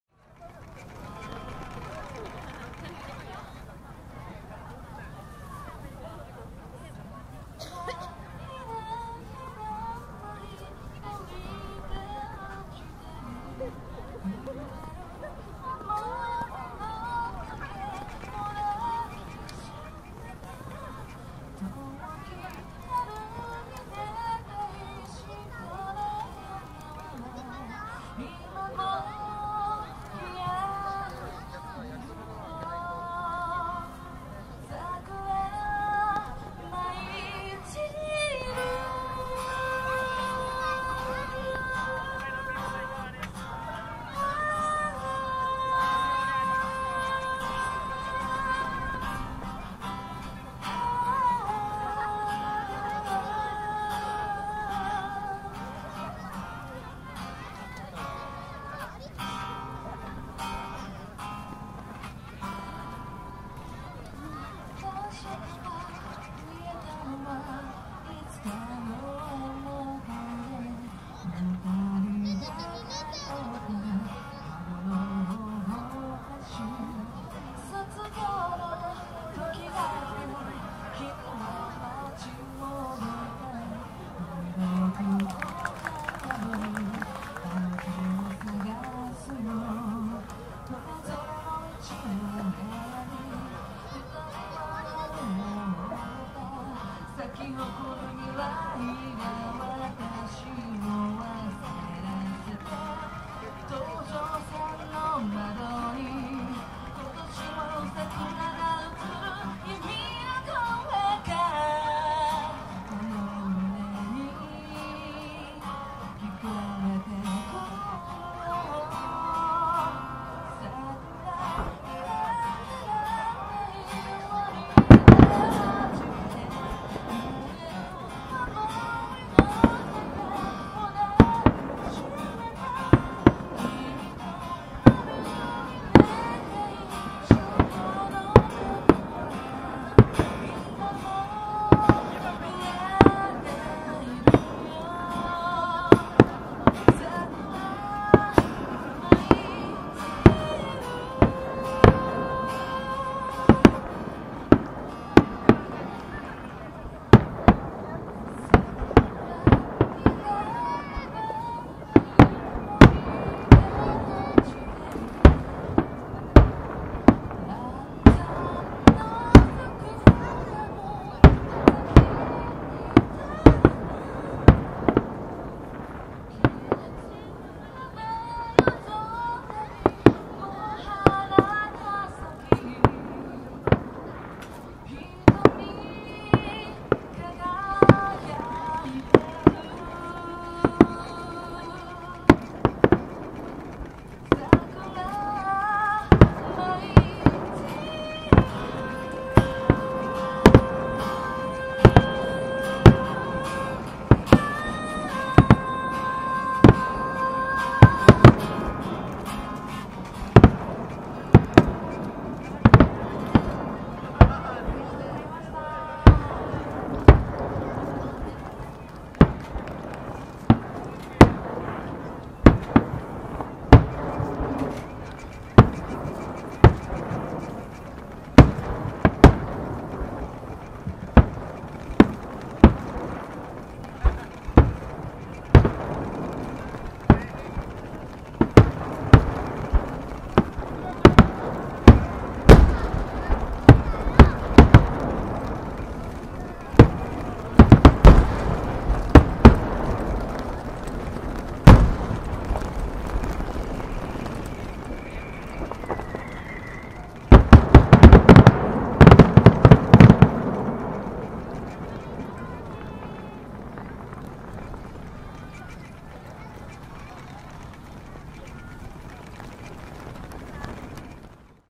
【2018】嵐山さくらまつり花火大会「フィナーレ」Ranzan Cherry Blossom Festival fireworks
2018年4月7日埼玉県嵐山町にて行われた花火大会
ギターとボーカルによるアコースティックライブとコラボの花火
最初はライブのみで花火が上がらなかったので、
最初の録画ミスで音が撮れてなかった映像を入れましたw

花火師：神田花火